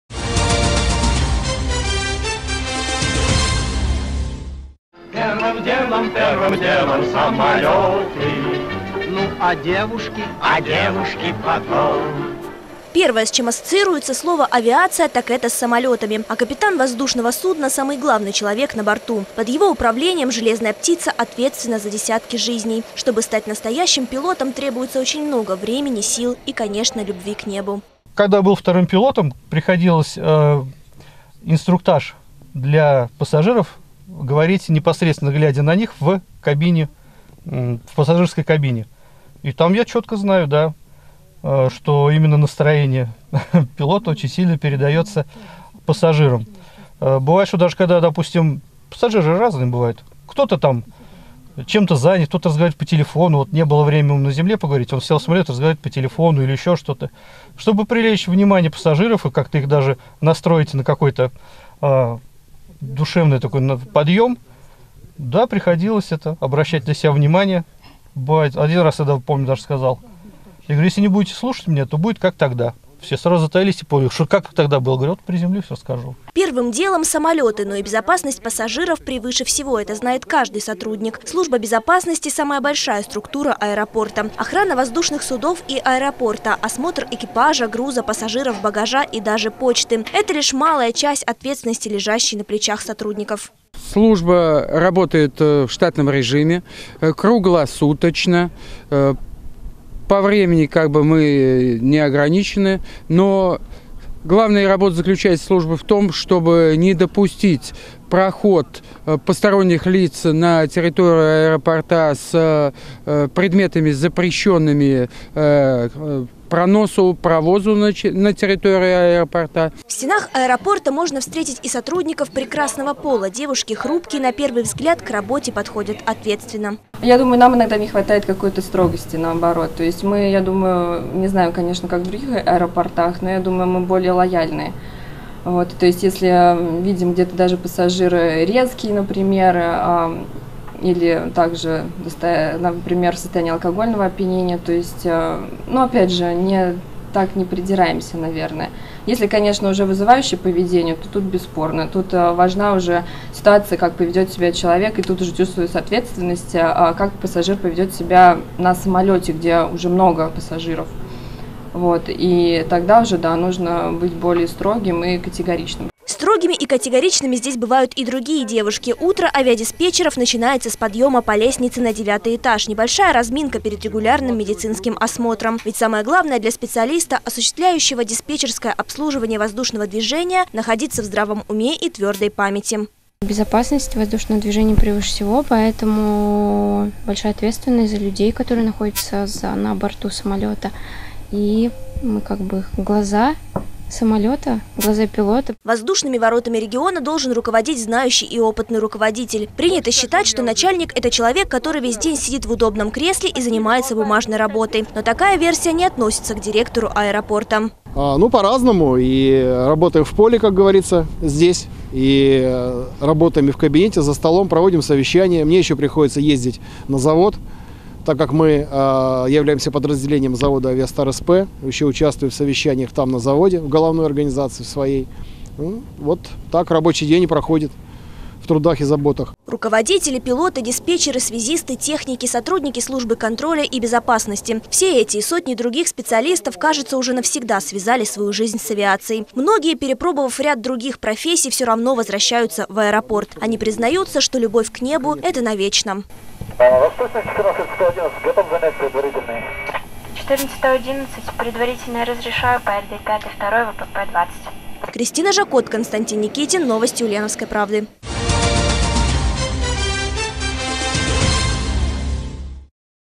С любовью к небу. Как работают воздушные ворота Ульяновска
9 февраля в России отмечается День гражданской авиации. Первое, с чем ассоциируется слово «авиация», конечно, с самолетами. А капитан воздушного судна – самый главный человек на борту. И он несет ответственность за десятки жизней. Чтобы стать настоящим пилотом, требуется очень много времени, сил, и, конечно, любви к небу.